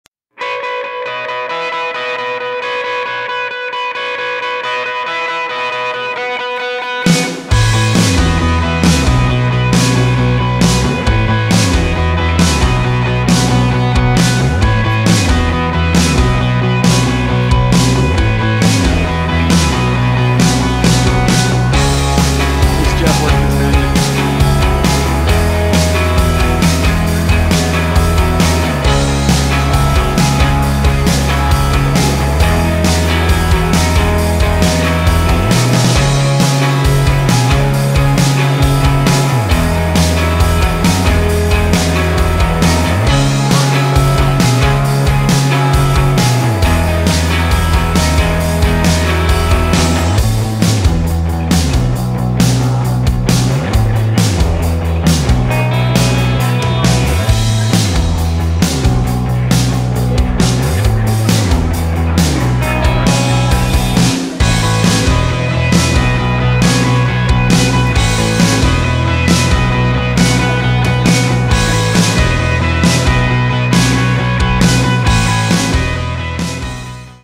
Watch these barbers do something amazing!
The Barbers and Stylists of Locals Barbershop at University Village in Colorado Springs volunteered at Community Prep School offering free hair cuts and styles to the kids and staff on CPS Count Day.